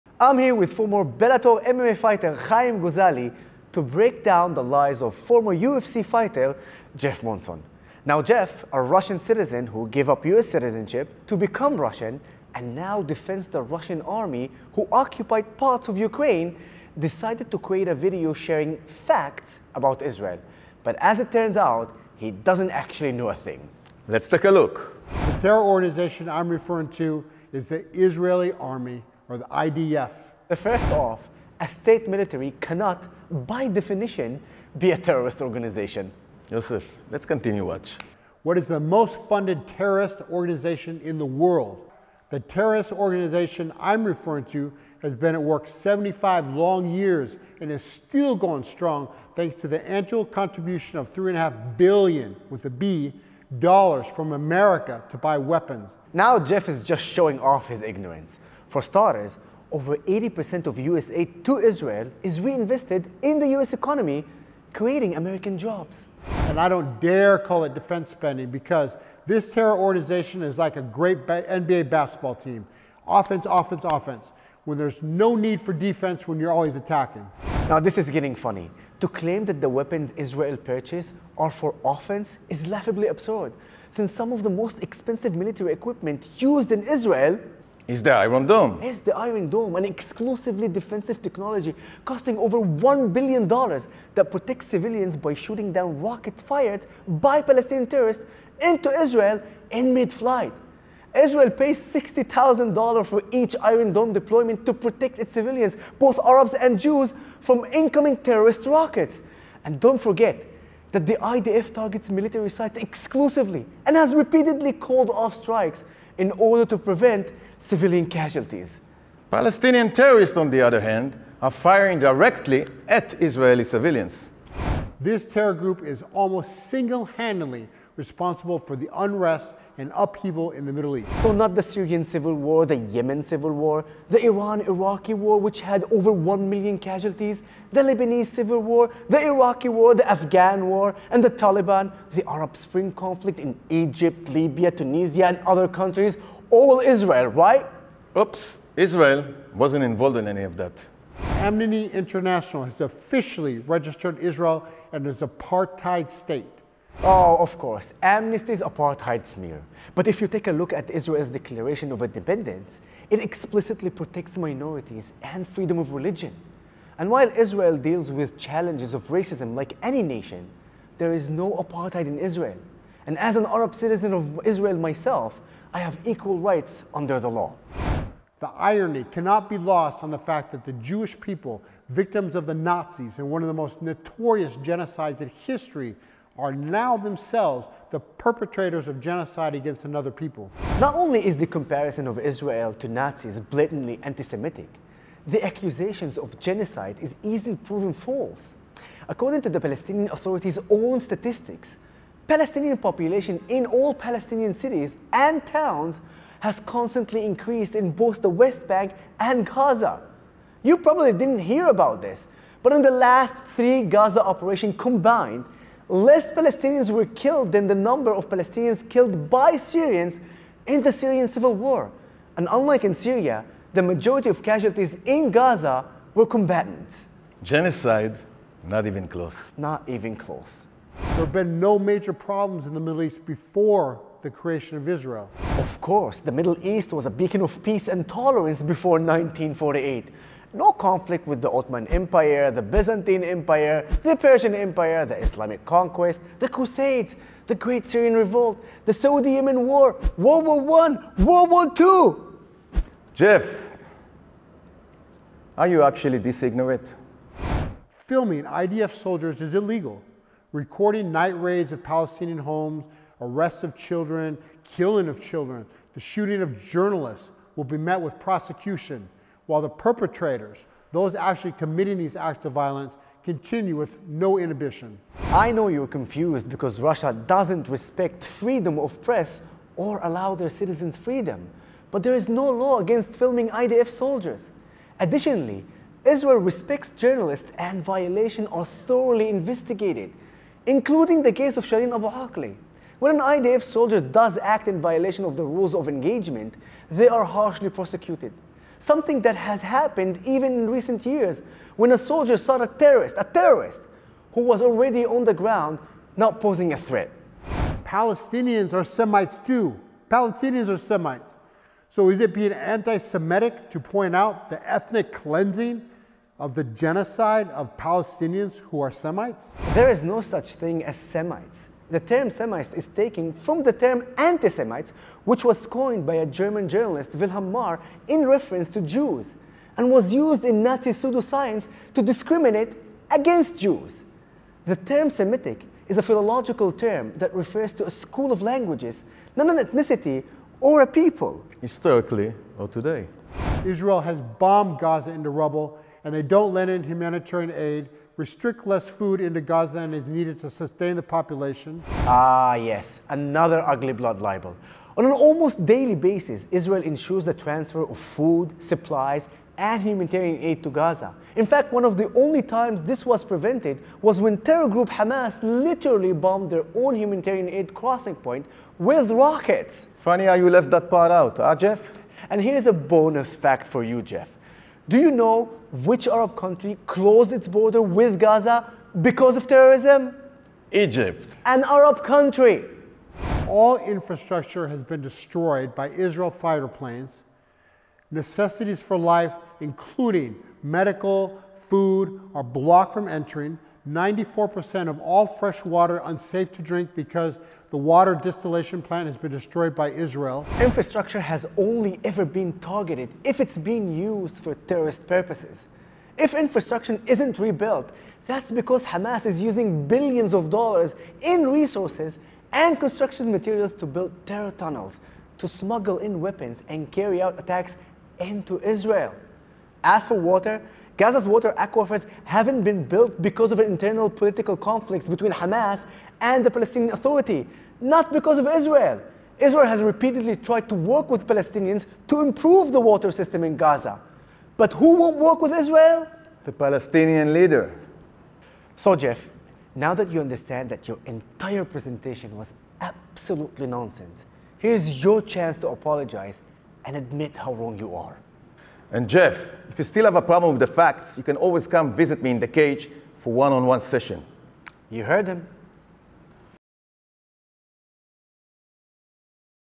Yoseph Haddad and MMA fighter Haim Gozali break down the lies of former UFC fighter Jeff Monson.
@-yosephhaddad9088  and former Israeli Bellator MMA fighter@HaimGozali1  break down the lies of former UFC fighter Jeff Monson
For more ⬇